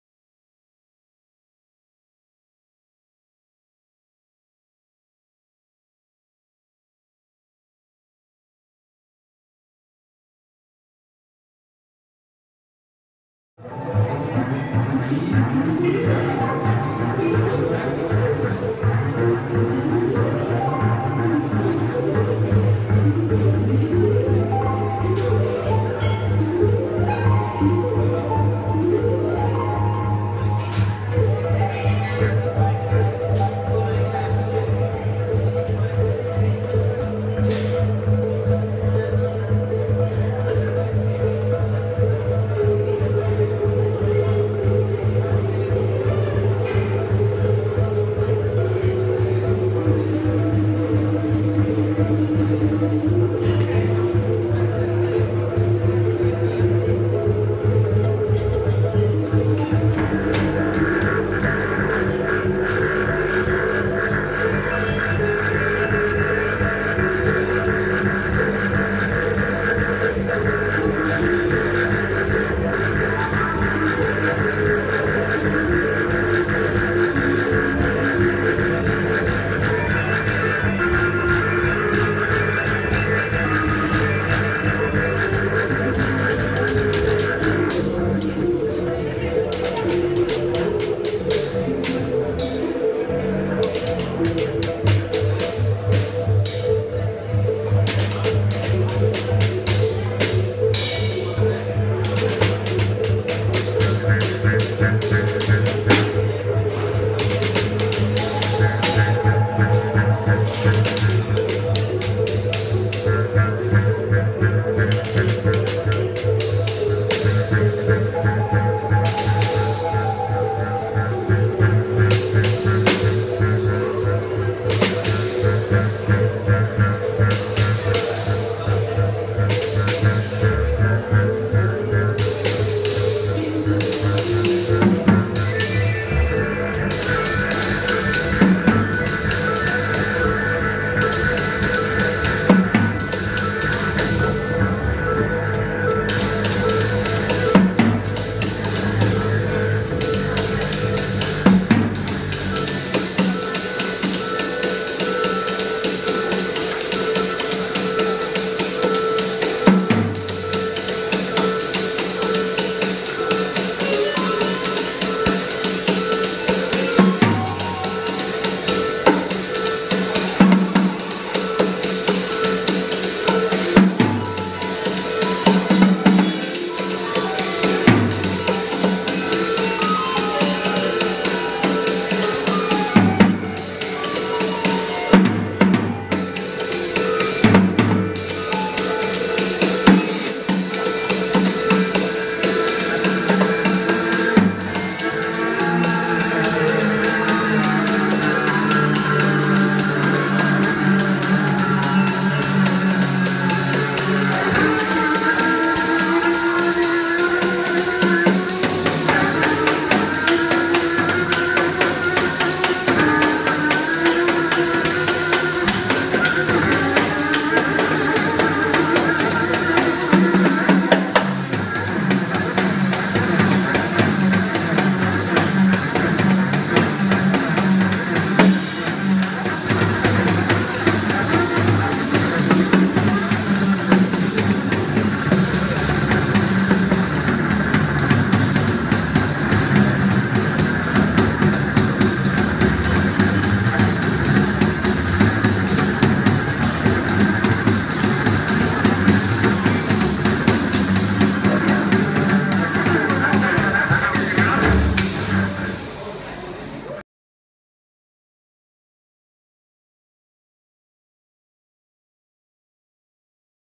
Dave De Rose, John Edwards, Dan Nicholls (1)
Extracts from Bedtime Stories episode 1. The first of a series of concerts presented by  Dave De Rose and The Agile Rabbit pizzeria in Brixton Market in April 2015.